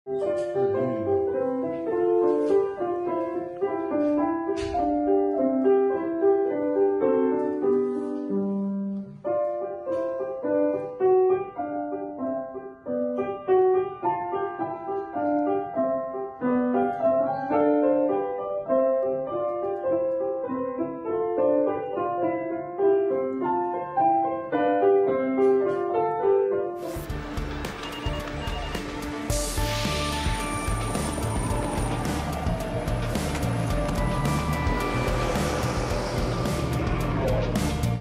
El día en que Nelson Castro "le robó" el piano al maestro Daniel Barenboim
El pianista y director argentino Daniel Barenboim tocó con la Filarmónica de Berlín en la Puerta de Brandenburgo para celebrar los 30 años de la reunificación alemana. Antes, una "perlita": Nelson Castro se sentó al piano del maestro Barenboim.